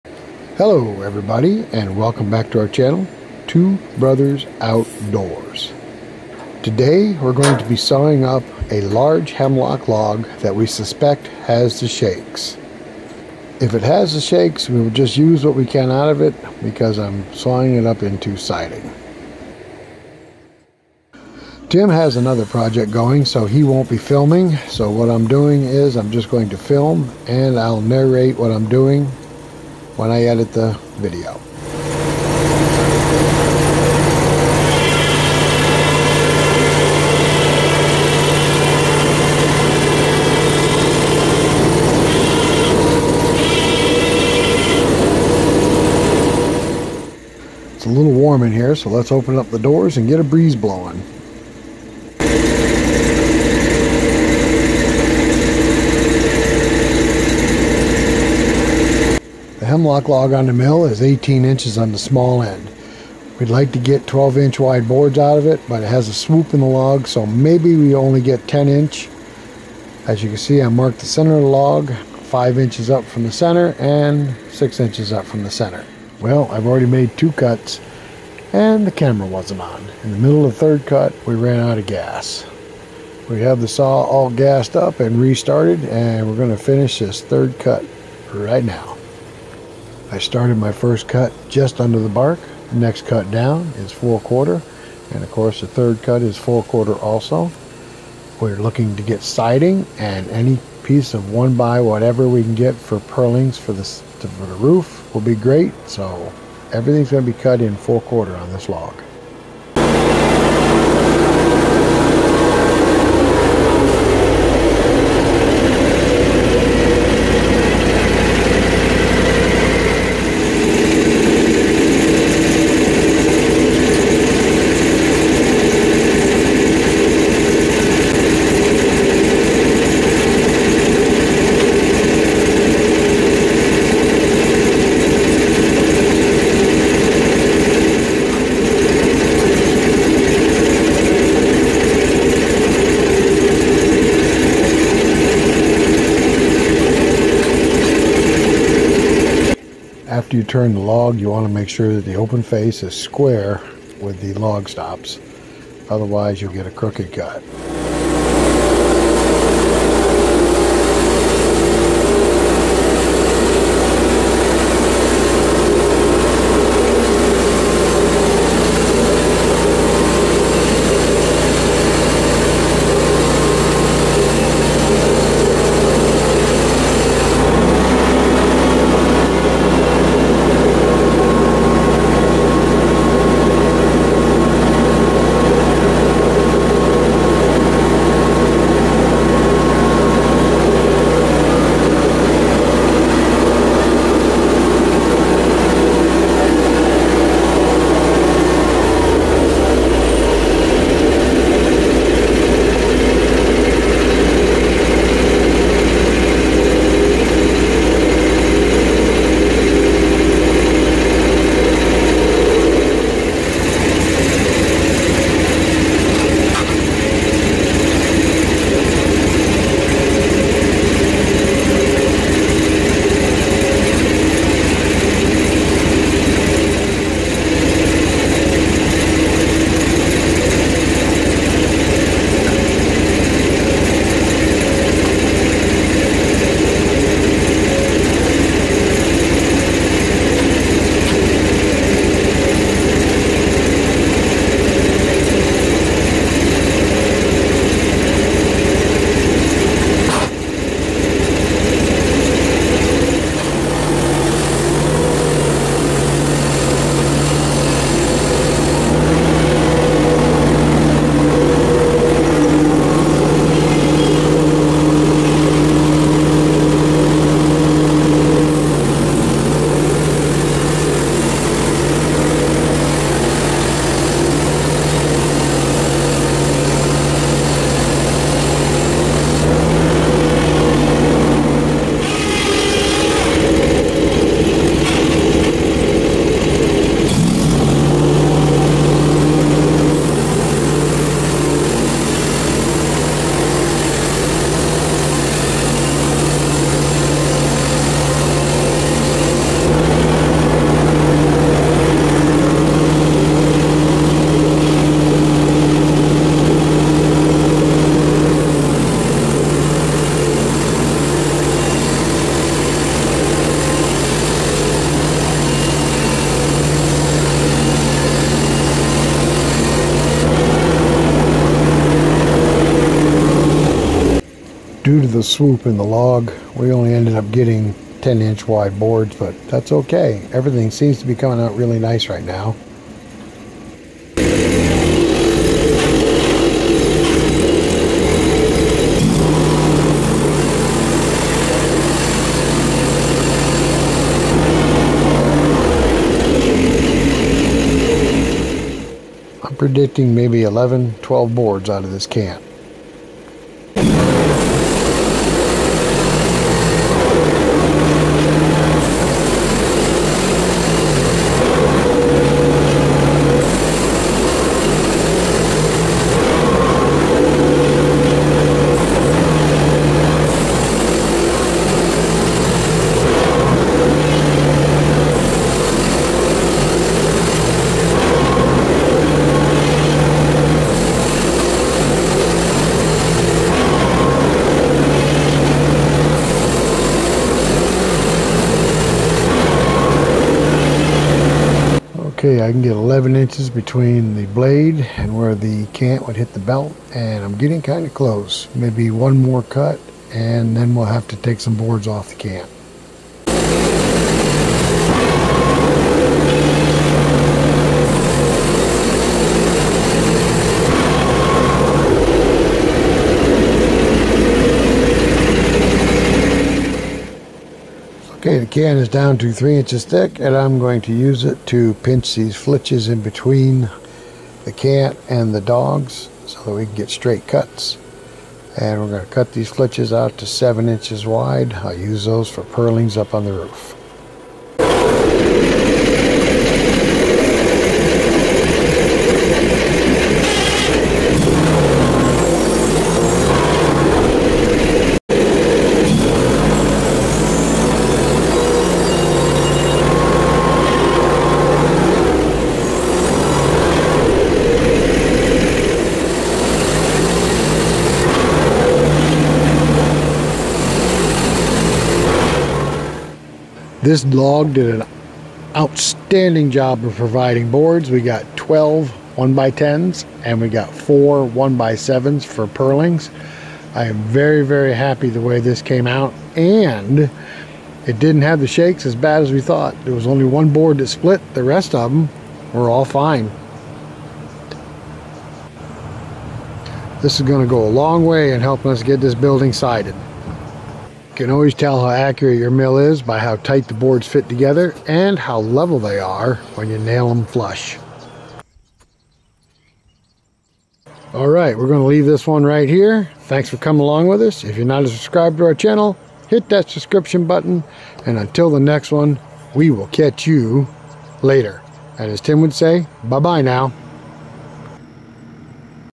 Bad Sign in Hemlock hemlock hudson sawmill
The black fungus rings in a hemlock tree is a sure sign of the shakes. this renders a log useless. I refuse to give up on this log. Did I salvage enough lumber to make it worth our efforts?

Check out: BASE CAMP® M Pro Dust Mask!